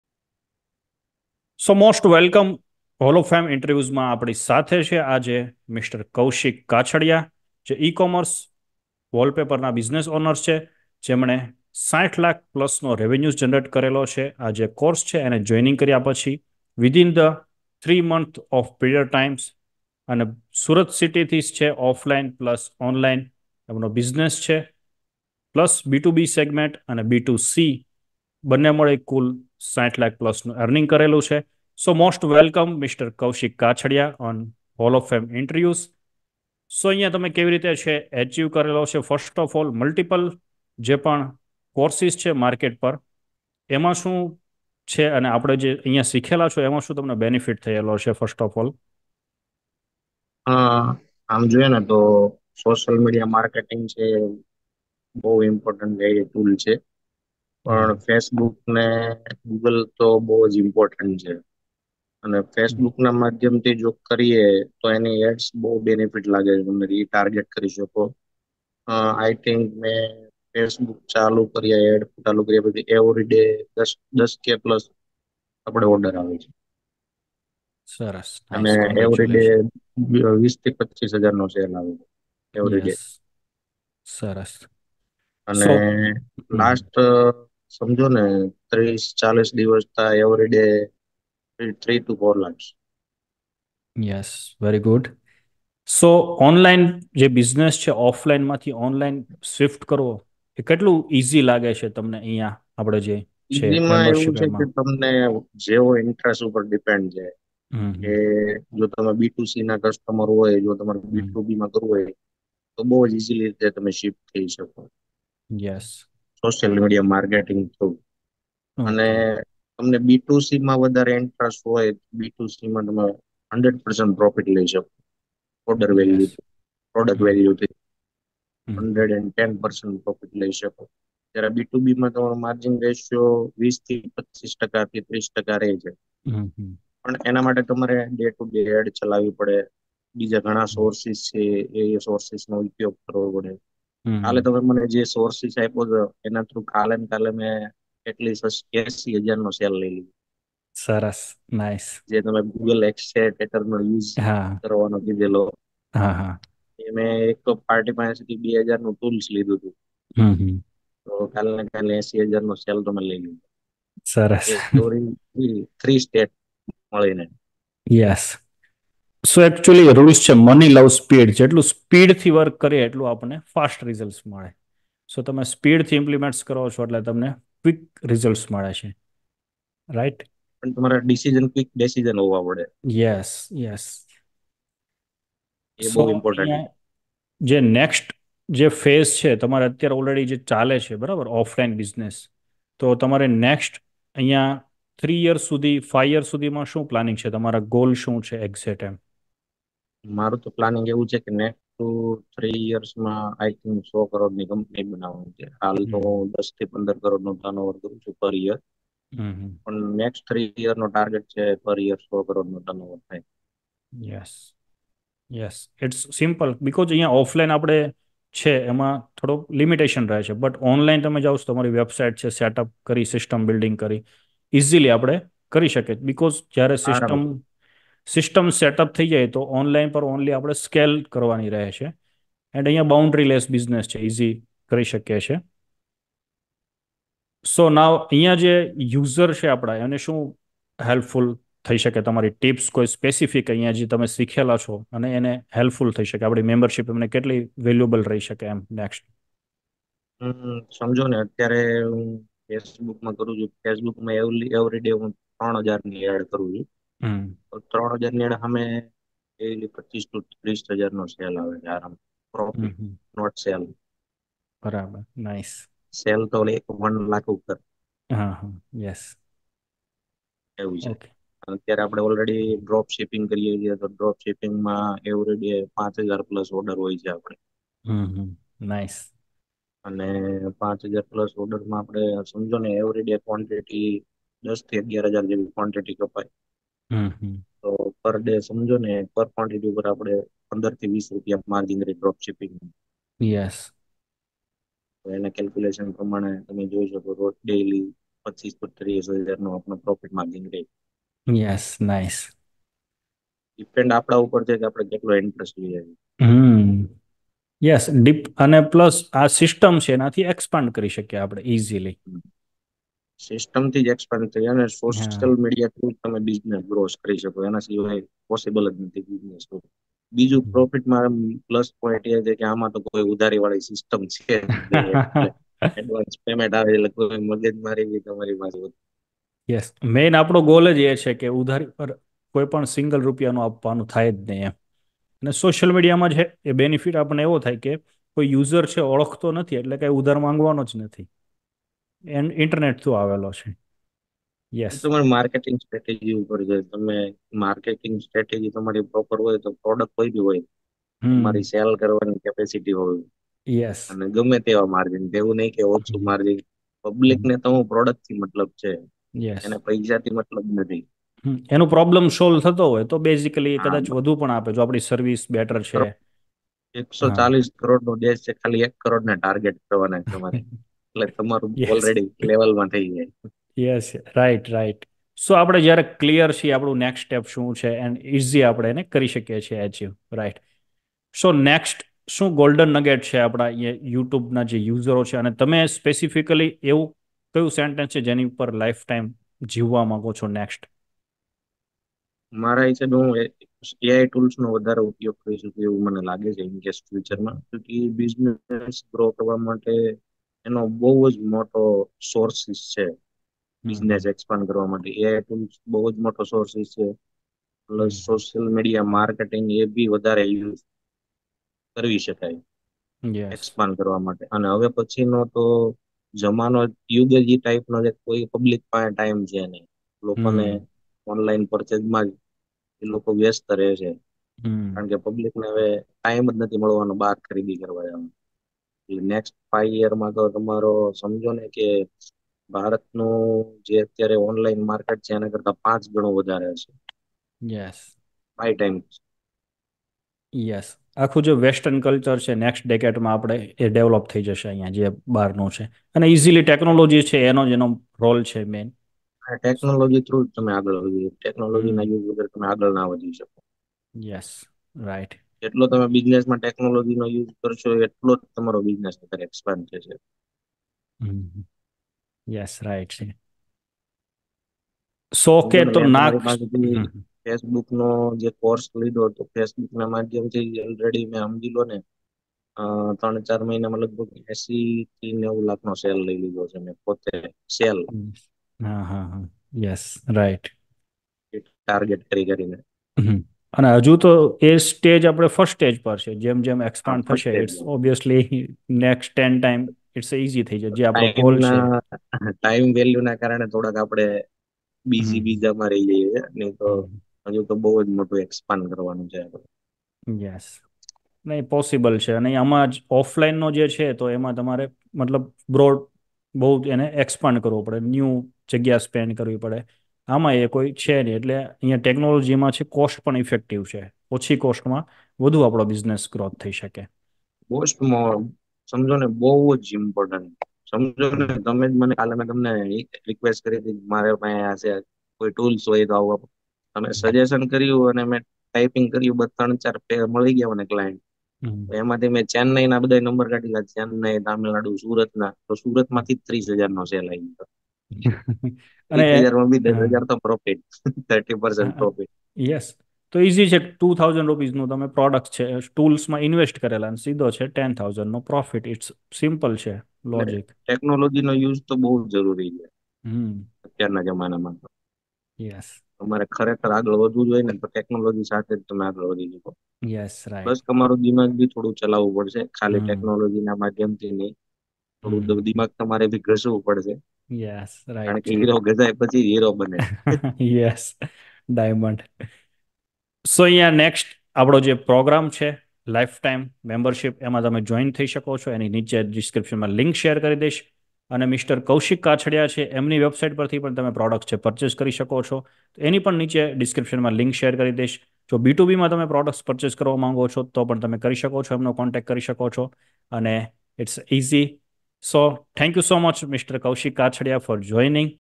Wall Paper Business Owner Kaushik Kachhadiya Achieves ₹60 Lacs In 3 Months
👉In this exclusive interview, we sit down with an extraordinary entrepreneur from our community who has achieved an incredible milestone: generating ₹60,00,000 in just 3 months through his wallpaper business! 🌟

👉Join us as we dive deep into his journey, exploring the strategies and secrets behind his success in both B2B and B2C markets. From understanding the market needs to effective marketing tactics, you'll gain valuable insights that can help you elevate your own business.

✅ Income - ₹60,00,000 in 3 months
✅ Designation - Wall Paper - Owner
✅ Based In - Surat